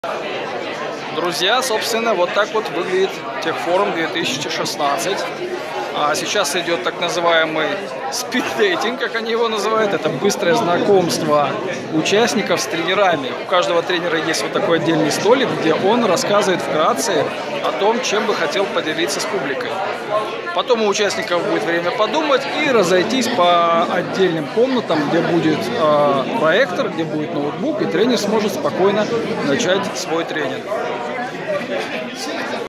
В чем суть астанинского Techforum 2016: первые часы работы
На это великолепное мероприятие, прошедшее 6-8 июня в Астане, я привез свой самый, пожалуй, востребованный трениг, посвященный созданию сложных мультимедийных постов. Народу было - не протолкнуться! Говорили на английском, русском, казахском, туркменском, узбекском и таджикском языках. В общей сложности собралось примерно 70 молодых ребят-журналистов и пиарщиков, плюс еще с десяток тренеров. Это был настоящий форум, с хорошей организацией и ясной поставленными целями.